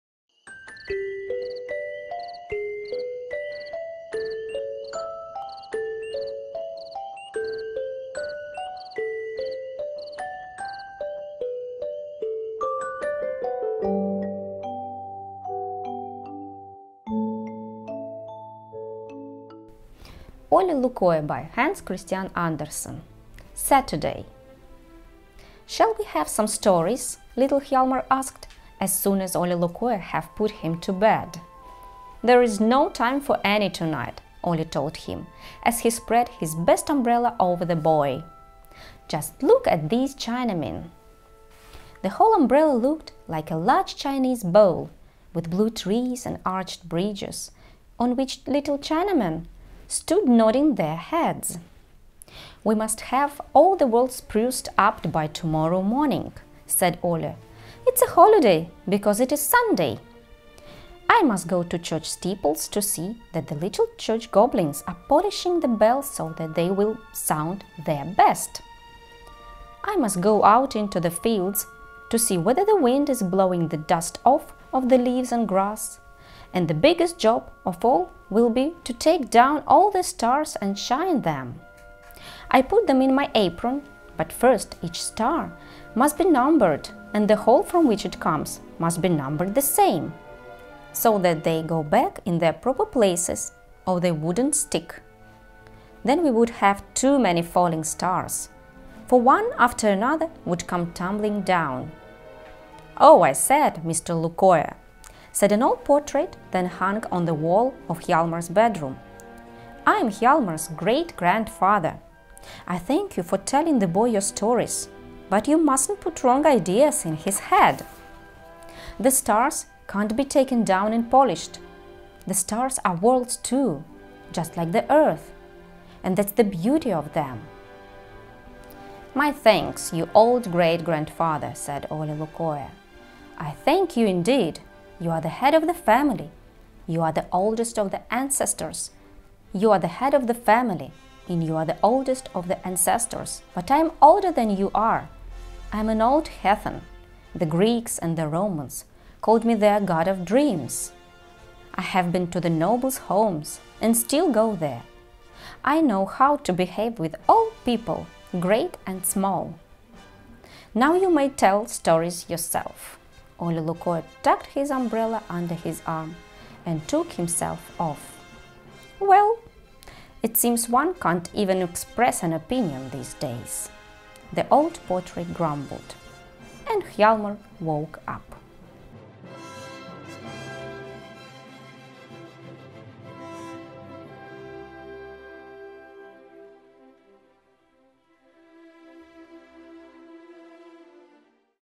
Нина Элбакиева читает часть "суббота" сказки "Оле Лукое" Ханса Кристиана Андерсена (англ)
Проект Читаем детям подготовил сюрприз для наших зрителей от 12 лет и их родителей. Мы публикуем сказку Оле Лукое на английском языке. Она будет без субтитров и рассчитана на тех ребят и их мам и пап, кто уже на хорошем уровне знает английский язык и готов слушать сказки без субтитров.

Кроме этого мы объявляем конкурс на лучший перевод (субтитры к ролику могут добавлять все пользователи). По итогам мы выберем лучшие субтитры и подарим их автору приятный приз от театра Кот Вильям.